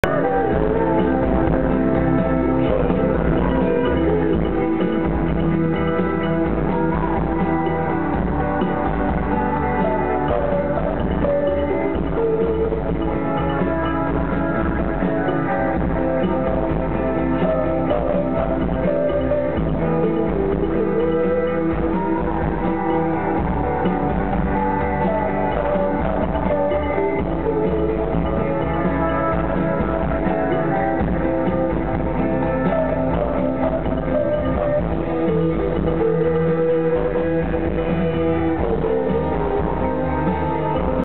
Blonde Redhead at ACL 2007
A snippet of Blonde Redhead at the Austin City Limits Music Festival 2007